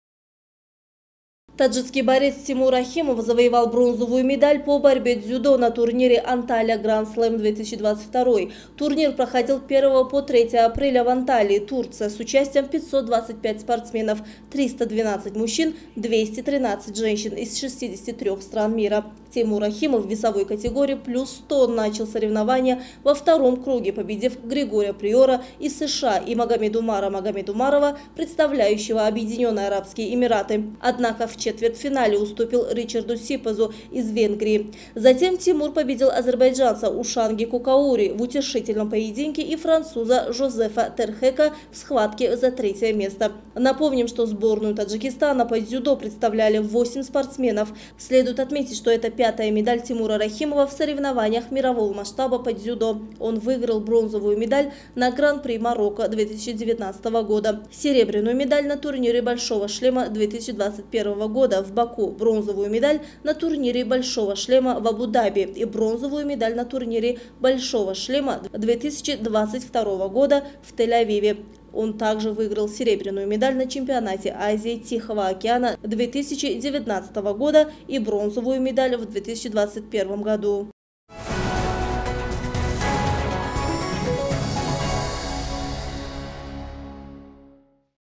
Дзюдо «Antalya Grand Slam 2022»
На турнире по дзюдо «Antalya Grand Slam 2022», который проходил с 1 по 3 апреля в турецкой Анталии, таджикский борец Темур Рахимов завоевал бронзовую медаль. На турнире принимали участие 525 спортсменов из  63-х стран мира.